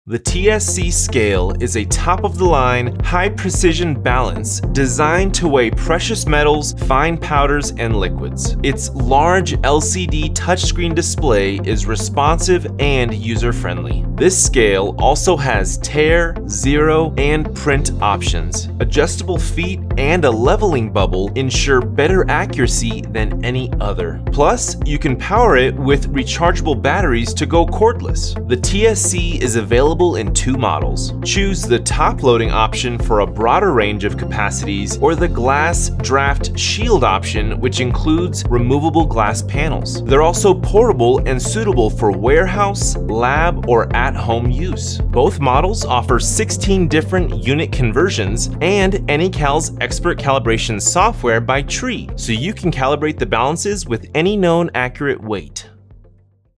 TSC (Touchscreen High Resolution Balance)
Check out the Tree brand Touchscreen balances.  TSC Series touch screen are high resolution balances with 14 switchable weighing units. Easy to use and matches the precision of balances that are 2 to 3 times the cost. It is designed without unnecessary bells and whistles.  The TSC features a large, backlit full color LCD touch screen display.  Software is simple to use, and calibration is menu driven.  It includes general weighing, counting, percentage weighing and density function.  It will be a new workhorse for any laboratory.  Built with additional features like auto backlight, auto shutoff & low battery indication.  The weighing pan is 7.68” x 5.9”.  The RS-232C & USB ports allow the TSC to interface to your PC.